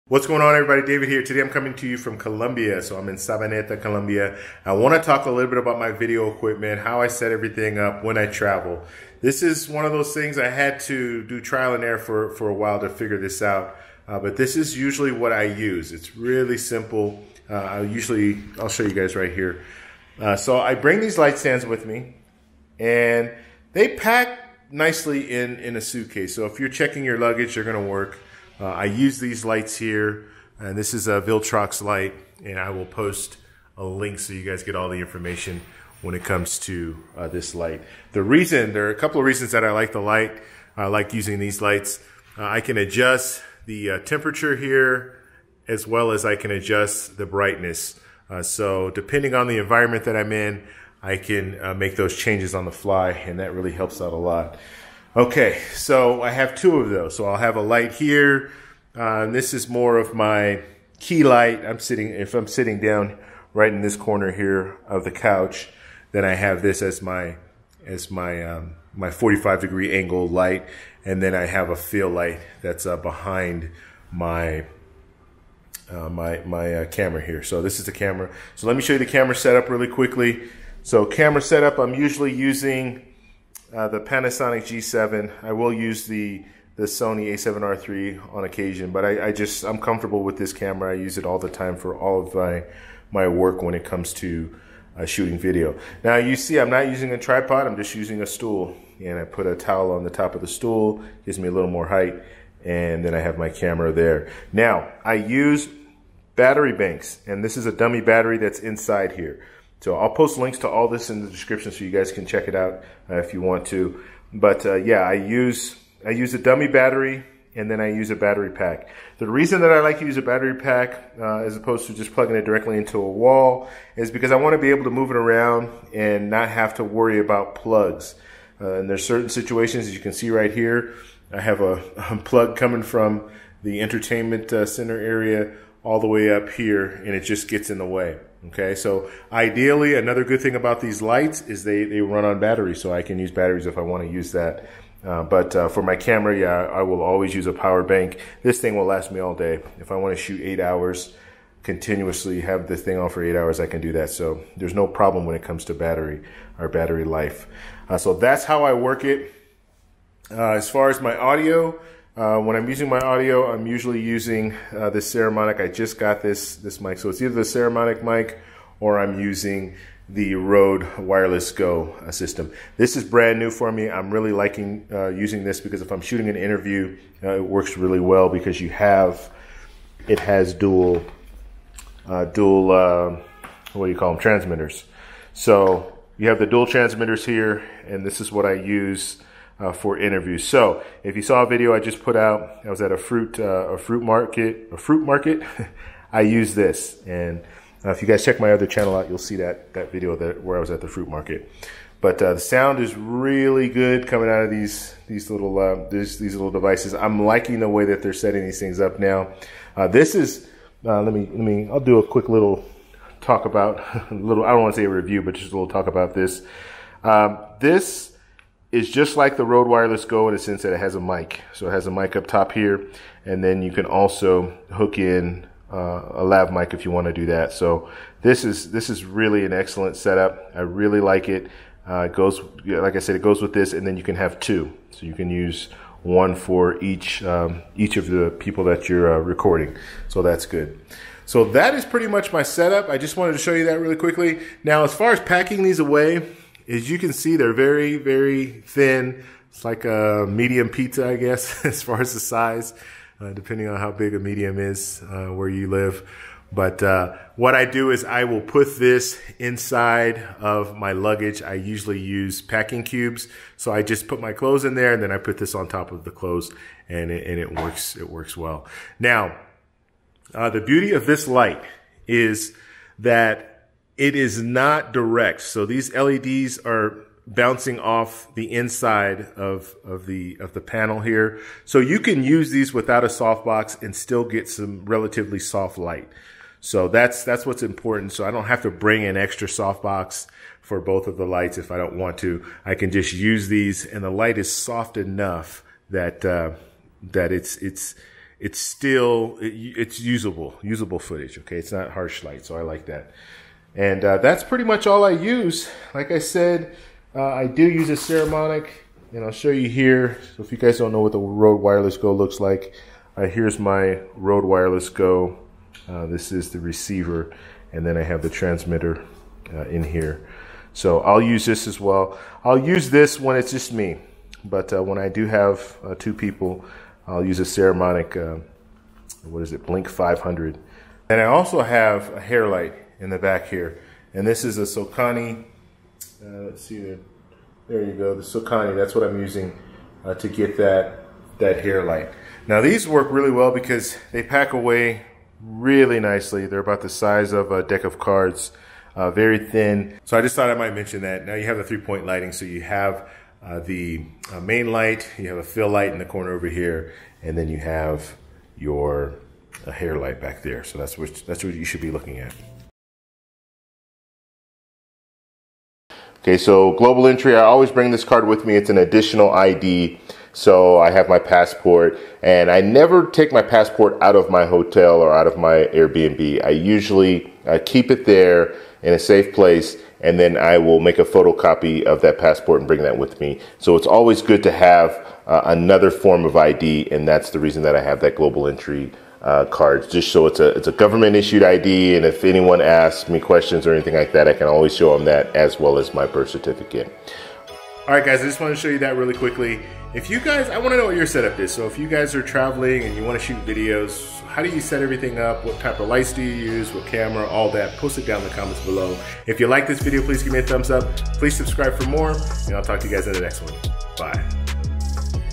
Youtube Equipment List for Making Videos (2019)
Check out the complete YouTube Equipment Checklist for Making  Videos on Kit here.  In this video I show you how to make videos on a budget.  I show you how to record a video step by step.   This is a quick and easy guide for beginners.   We also talk about how the right camera and audio equipment can help grow your youtube channel.  This is an on the go youtube setup for travel.